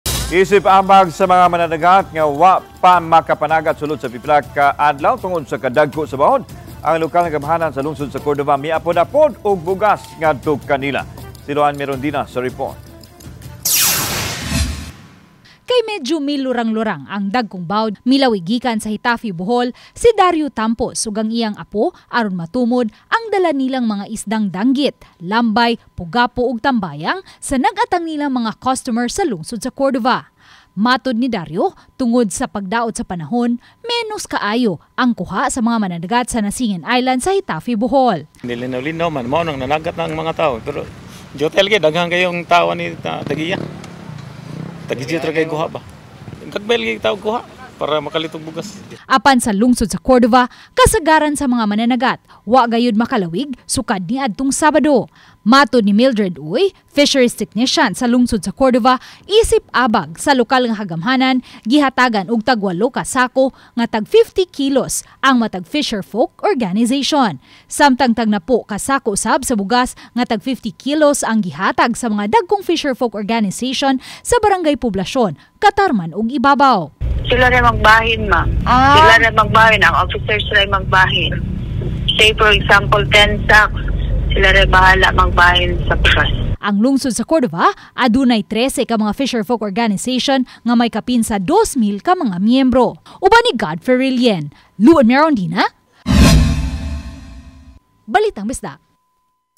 Balitang Bisdak: Mga mananagat sa Cordova nga wa makapanagat sulod sa pipila ka adlaw, giabagan
Isip abag sa mga mananagat nga wa makapanagat sulod sa pipila ka adlaw tungod sa kadagko sa bawod, lokal nga kagamhanan sa lungsod sa Cordova, miapod-apod og bugas ngadto kanila.